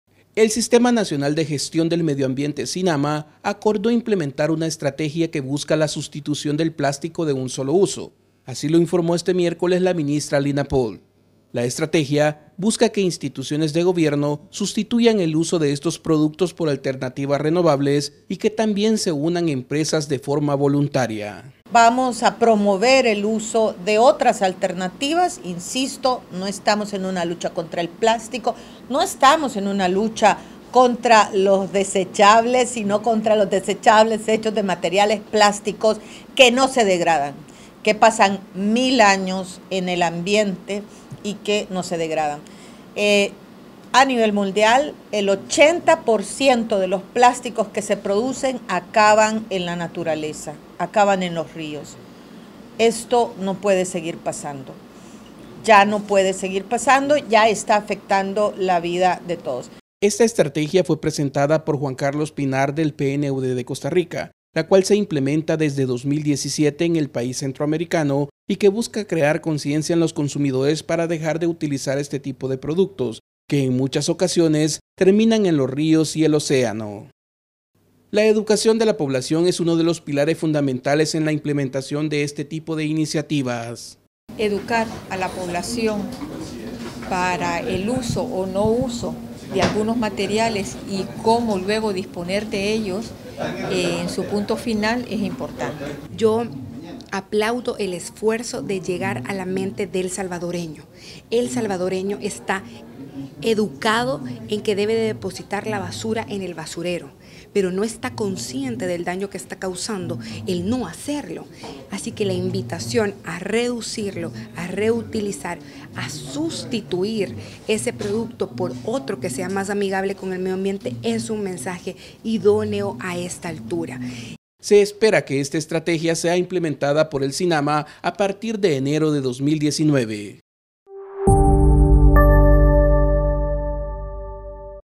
SINAMA impulsará sustitución de plásticos de un solo uso
los plásticos de un solo uso son aquellos que se emplean una sola vez y son desechados. En muchos de los casos, la vida útil puede ser de segundos o minutos, como por ejemplo vajillas y cubiertos desechables, removedores de café (agitadores), pajillas, contenedores de alimentos de comida rápida, bolsas plásticas y otros productos fabricados con materiales no biodegradables.